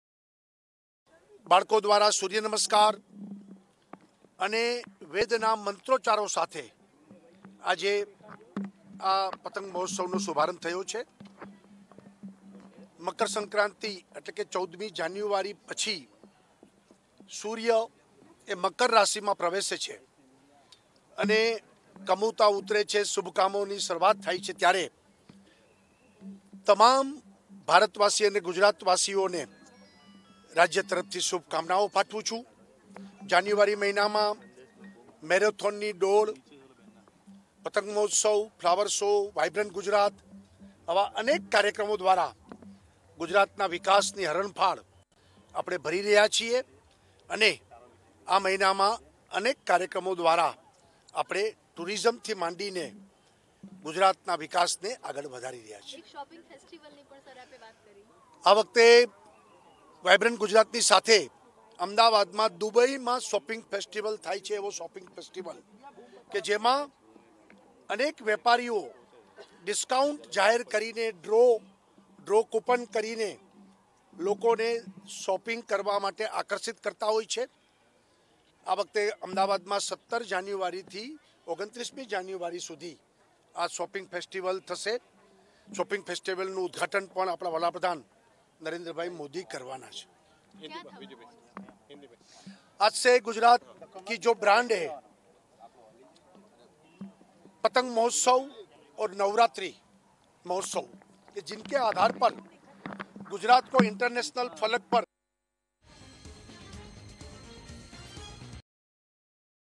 Ahmedabad:Gujarat Governor OP Kohli and state CM Vijay Rupani inaugurated the Kite festival
Ahmedabad:Gujarat Governor OP Kohli and state Chief Minister Vijay Rupani inaugurated the Kite festival